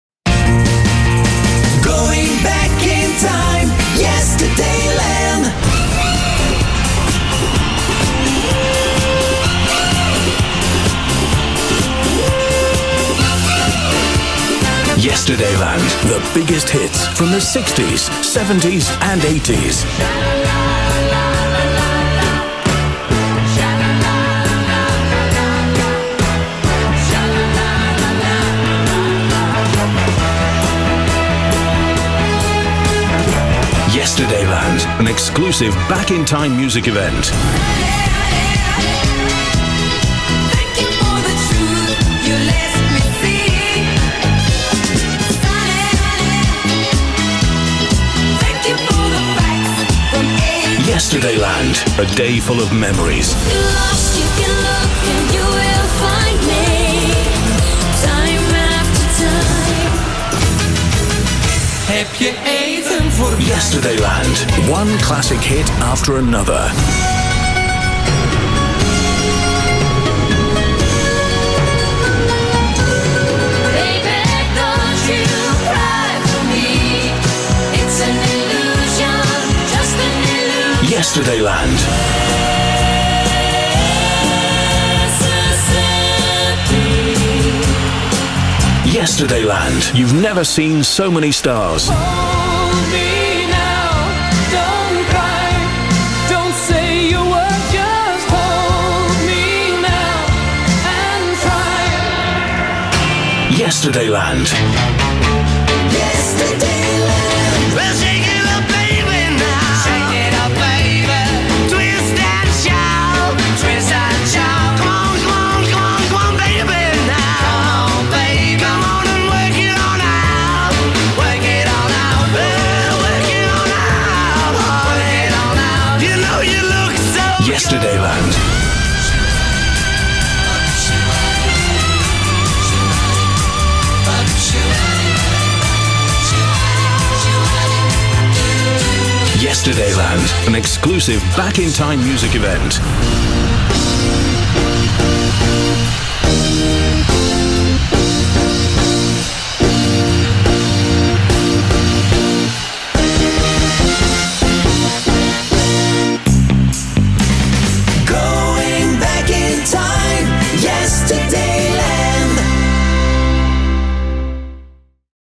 YESTERDAYLAND | The Aftermovie
YESTERDAYLAND… HET LAND VAN RETRO!

Meer dan 8.000 enthousiaste YESTERDAYLAND-bezoekers konden op 27 augustus in het sfeervolle en supergezellige Bobbejaanland genieten van het beste wat de 60s, 70s en 80s muzikaal achter gelaten hebben. Op liefst 7 podia was het genieten van live bands, top artiesten van toen en speciale shows die voor YESTERDAYLAND werden geproduceerd.

Met ondermeer The Fortunes, de legendarische Troggs en een special rond Elvis Presley was het genieten aan het 60s podium. Met een resem internationale topnamen als Pussycat, The Rubettes, Clout en BZN’s Jan & Anny was het meezingen aan de 70s stage terwijl Johny Logan en Ellen Foley dé internationale topnamen waren op het podium met 80s music!

Heel wat retrofiguren, tribute bands en special events, her en der verspreid over het pretpark, zorgden voor een unieke sfeer en nooit eerder vertoonde mix van shows en concerten. De Veronica Drive In Show, Ladies Of Pop met de Zuid Afrikaanse Nádine, Crooner Corner en daar waar waar Bobbejaan Schoepen zijn volk entertainde, zijn theaterzaal, zorgden Bobby Setter, Carry Goossens, Jan De Smet en Barbara Dex dat het hitverleden herleefde!

Een fantastische beleving voor elke bezoeker, van 10u00 tot 22u00. Afspraak in 2018 op zondag 26 augustus! Alle info via facebook en yesterdayland.be

ARTIESTEN & BANDS
60s stage : UB2 Big Band & Yannick Bovy – The Fortunes – The Magic of The Beatles – The Troggs – Elvis Matters 70s stage : The Rubettes – BZN’s Jan & Anny - Tony Christie - Pussycat feat Toni Willé – Clout and the original Cindy Alter 80s stage : Tributes to Roxette, Queen, U2 & Status Quo - Ellen Foley – ABBA Gold - Ladies of Pop by Nádine ARENA :  Barry White tribute by Eric Conley – Her name is Amy Winehouse – Tribute to Michael Jackson – The best of The Bee Gees – Chiel Montagne – Luv – John Terra - Vader Abraham – Sam Gooris – Frans Bauer CROONER CORNER : Lisa Del Bo – Guy Neve VERONICA DRIVE IN SHOW rock classics HISTORY THEATRE : Fats Domino Special by Bobby Setter & his orchestra – Bobbejaan Schoepen Show met Barbara Dex & Jan De Smet – Ode aan Johnny Cash door Carry Goossens DEEJAY’S & PRESENTATOREN : Tom De Groot – Luc Van Mechelen – Benny Cobra – Jos Willems – Marc Huylebroeck – Peter Van Wyck – Jurgen Jonkers – Jurgen Roels